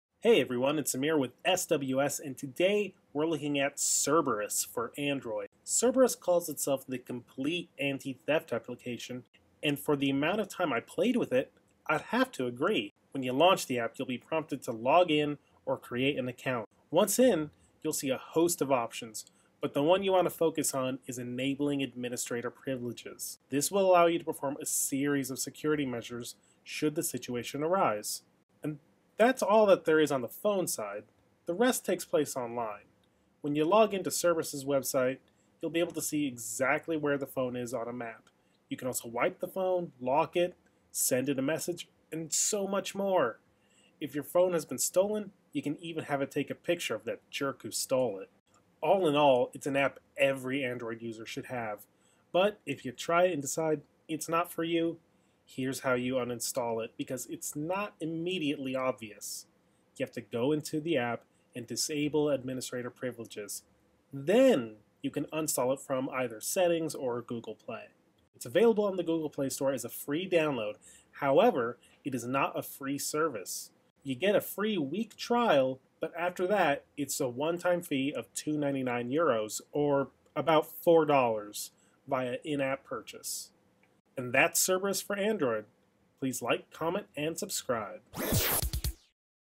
Cerberus - App Guide - SWS Computers
It's our first Android app, Cerberus, the best in anti-theft protection for Android. Got an Android app you would like to see us review?